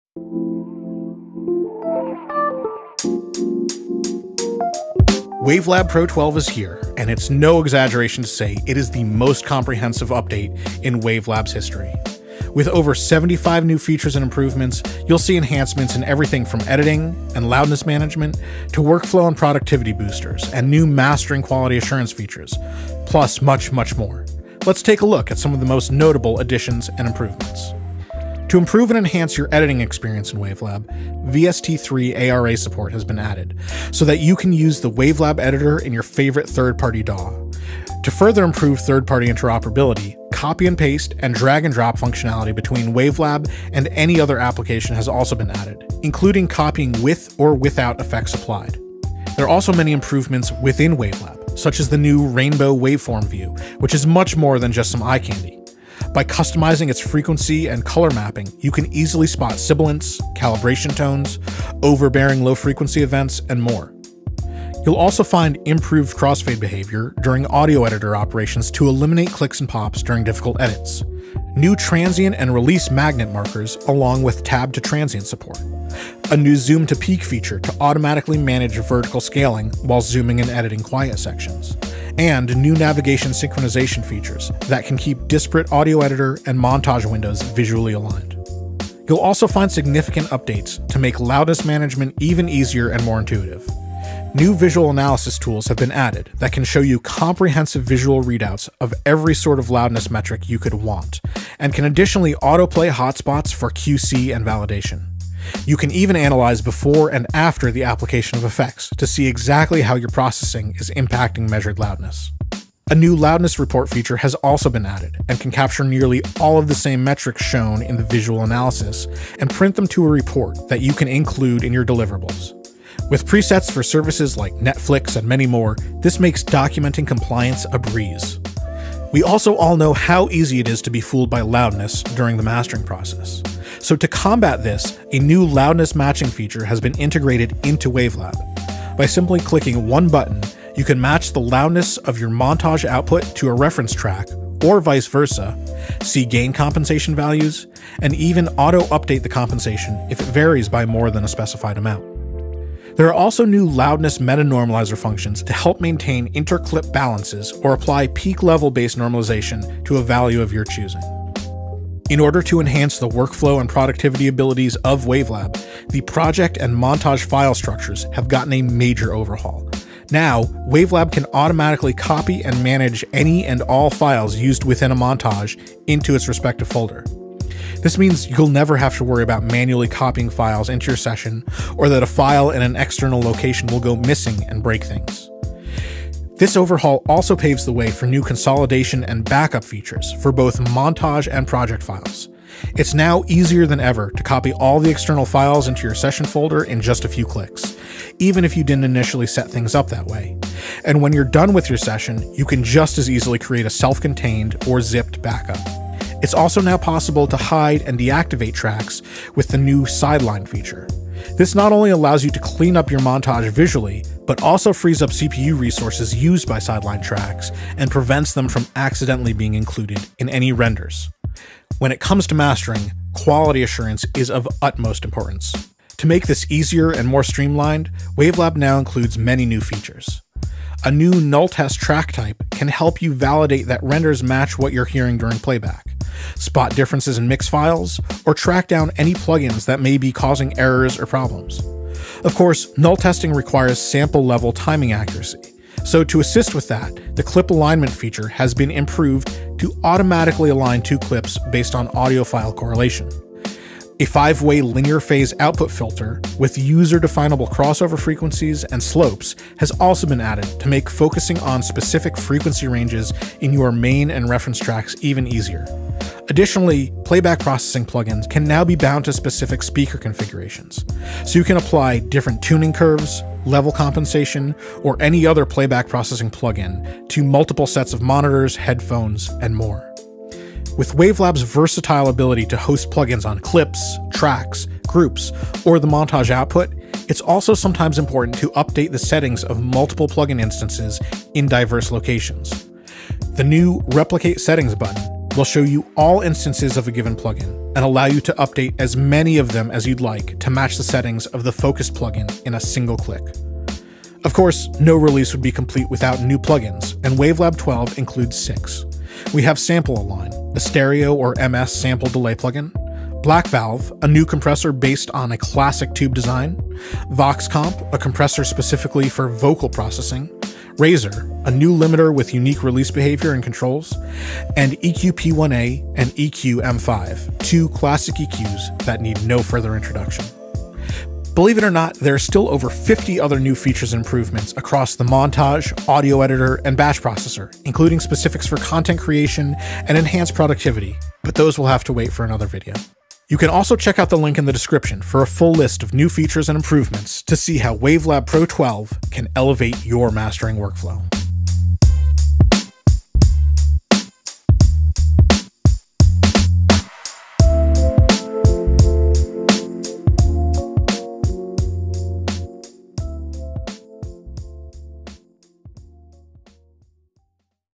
New Features in WaveLab 12 | Promo
Steinberg WaveLab 12 includes over 75 new features and improvements for audio editing, spectral editing, plug-in effect processing, workflow, productivity and audio mastering quality assurance. If you’re mastering for streaming or for vinyl and any other format, WaveLab 12 is your ultimate mastering DAW.